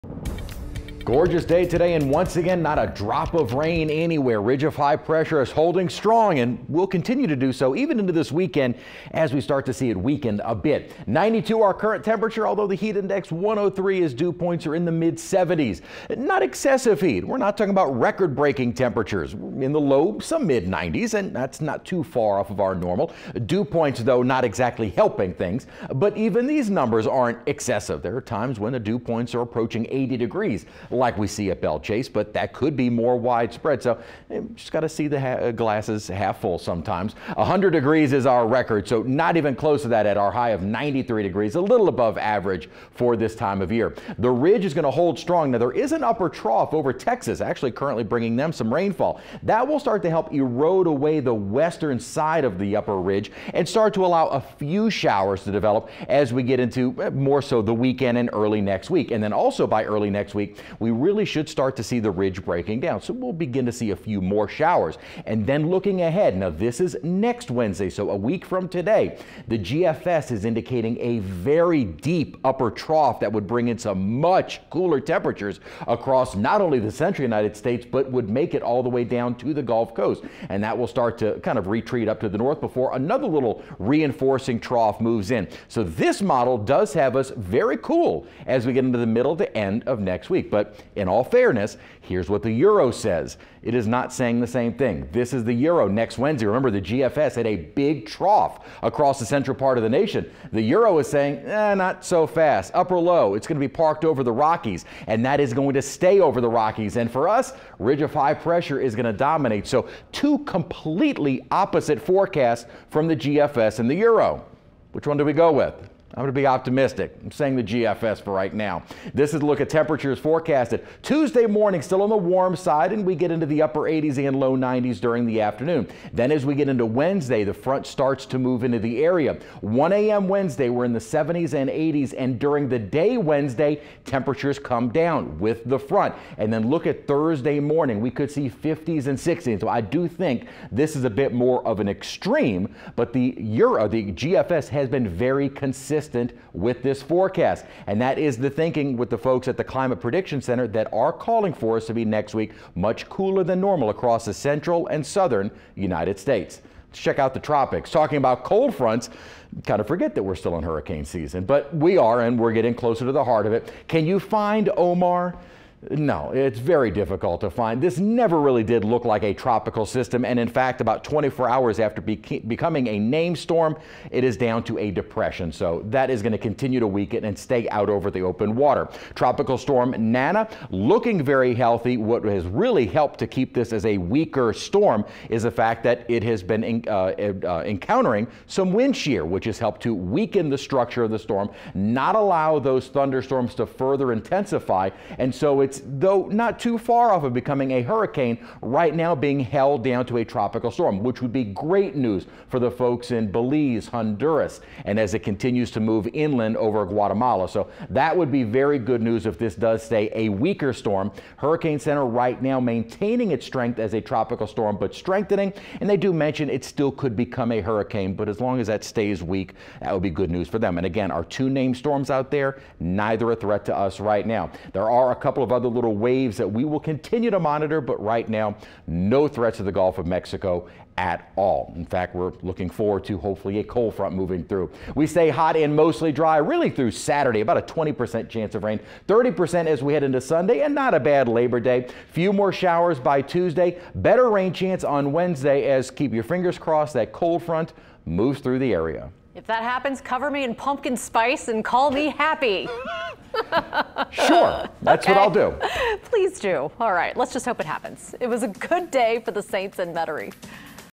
Weather: Hot and Dry This Week, Cold Front Next
Chief Meteorologist Chris Franklin has a look at a potential cold front next week and an update of the tropics.